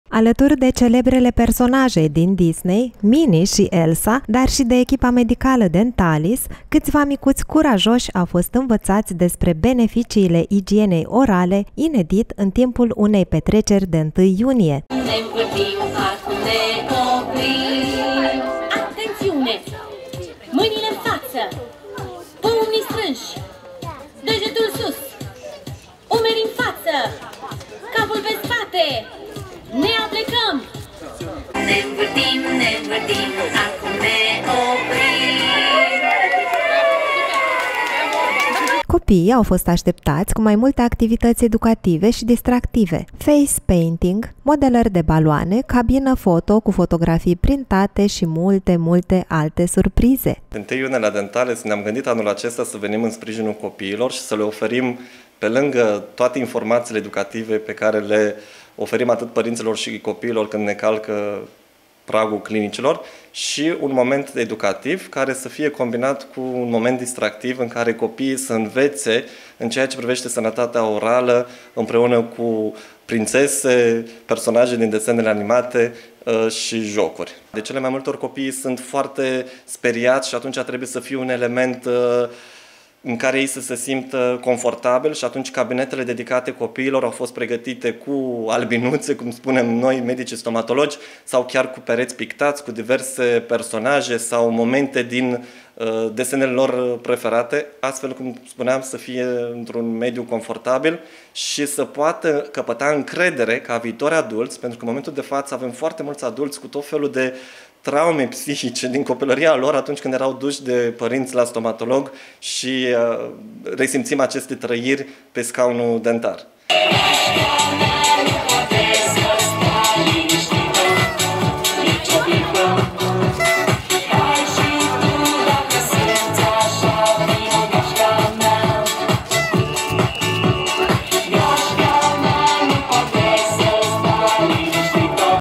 Distracție și educație, pe 1 iunie, la ”Dentalis”
Alături de celebrele personaje din Disney, Minnie și Elsa, dar și de echipa medicală Dentalis, câțiva micuți curajoși au fost învățați despre beneficiile igienei orale, inedit, în timpul unei petreceri de 1 Iunie. Copiii au fost așteptați cu mai multe activități educative și distractive: facepaiting, modelări de baloane, cabină foto cu fotografii printate și multe, multe alte suprize.